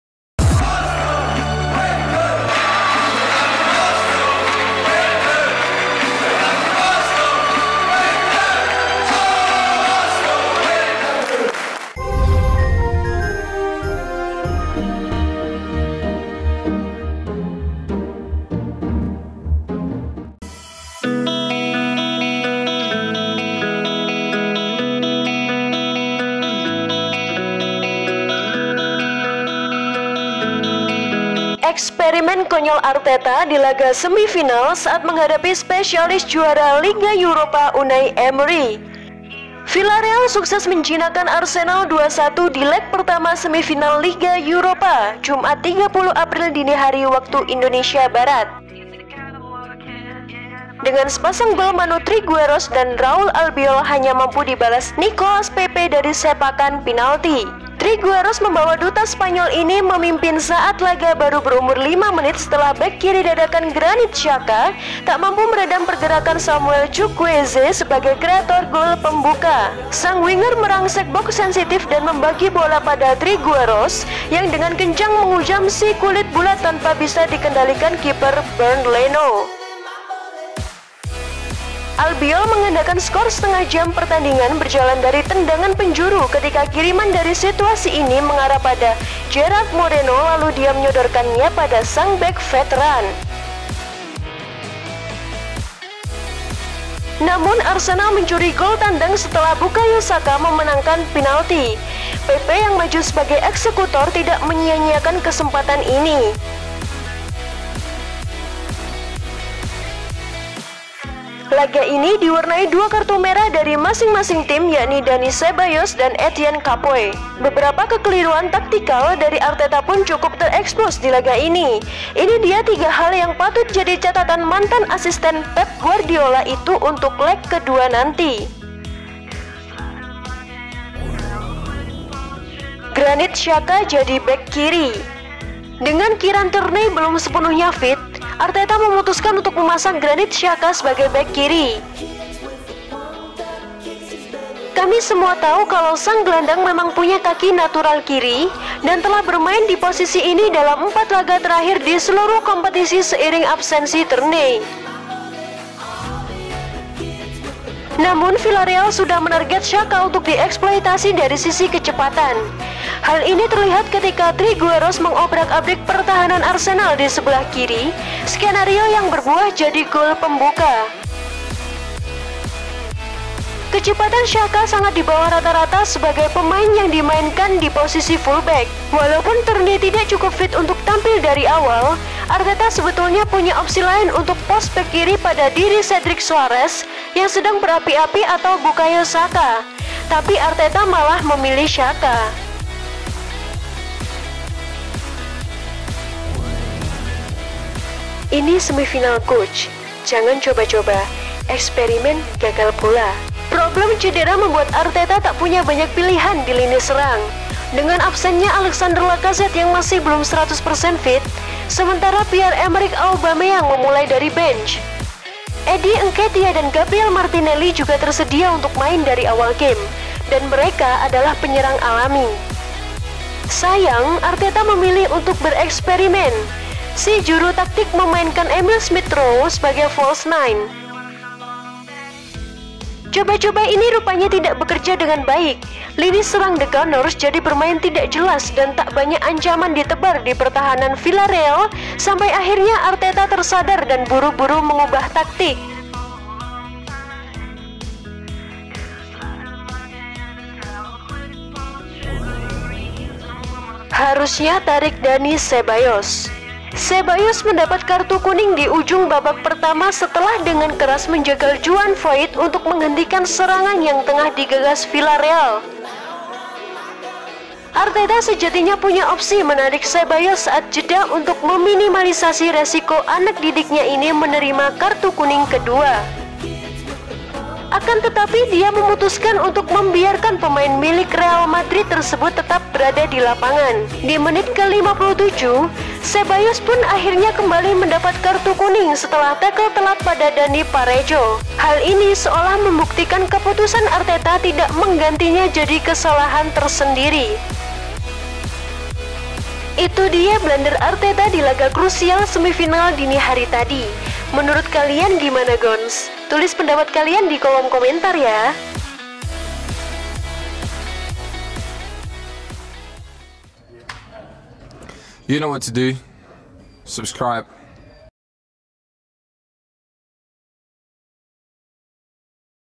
Please ARTETA Jangan Konyol di Laga Semi-Final saat Hadapi Unai Emery Spesialis Liga Europa
- Eksperimen konyol Arteta di laga semi-final saat menghadapi spesialis juara Liga Europa, Unai Emery.
- Granit Xhaka jadi bek kiri
- Ini Semi-final coach, jangan coba-coba? Eksperimen gagal pula!
- Harusnya tarik Dani Ceballos